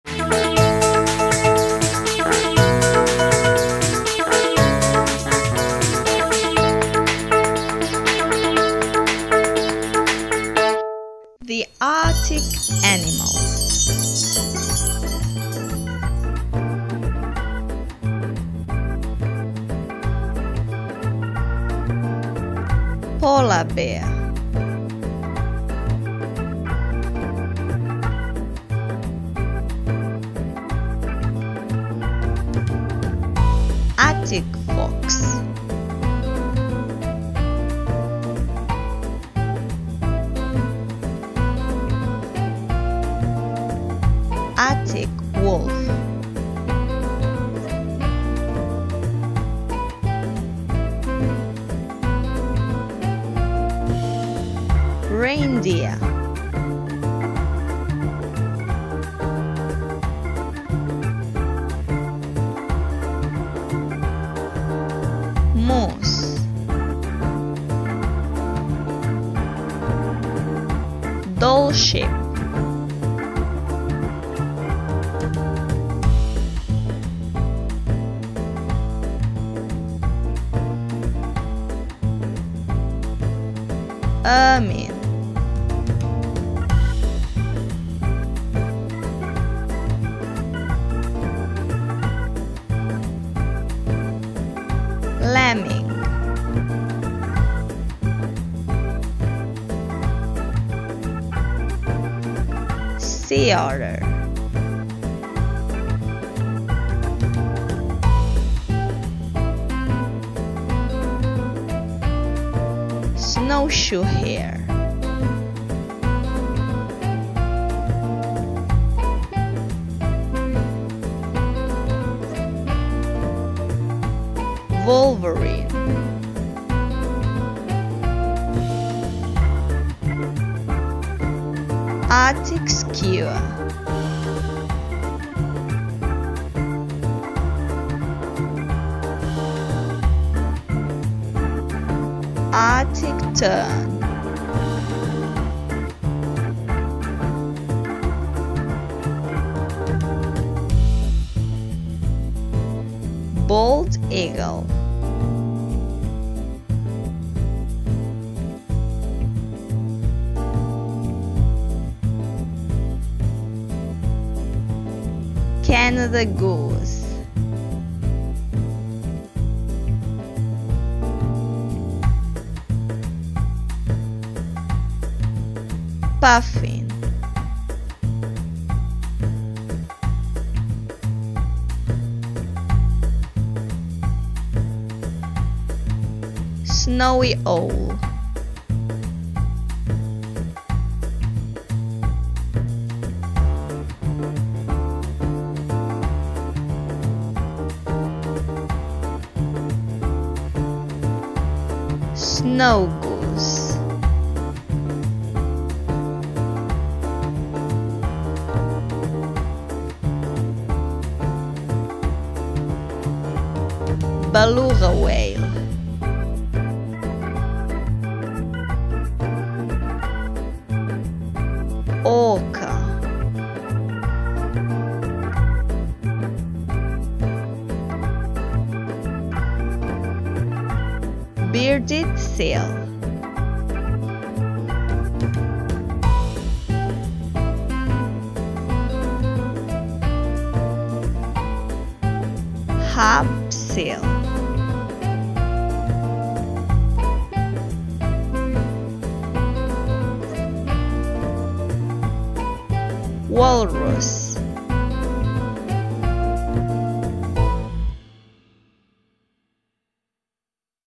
Arctic Animals for Children: pronunciation in English (with videos)
Welcome to the world of Arctic animals!

Check with your children and students the names of Arctic animals. Children will travel to the North Pole discovering the polar animals of the Arctic in English in a funny way, while watching the moving animals, hear it in English, and how to write and read.